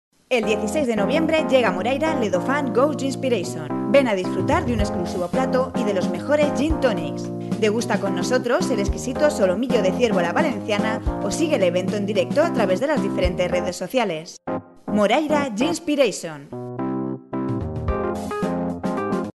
The best cuisine in Moraira close to the beach, in the restaurant of luxury Le Dauphin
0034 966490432

Luxury restaurant in Moraira, in the bay of El Portet. In Le Dauphin you can taste exquisite French - Mediterranean gastronomy in a privileged place, with a chill out zone and with wonderful landscape to the sea